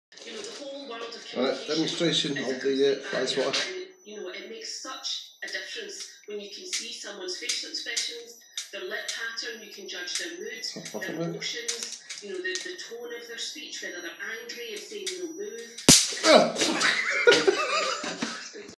Electric fly swatter demonstration
Thought the batteries had run out of the electric fly swatter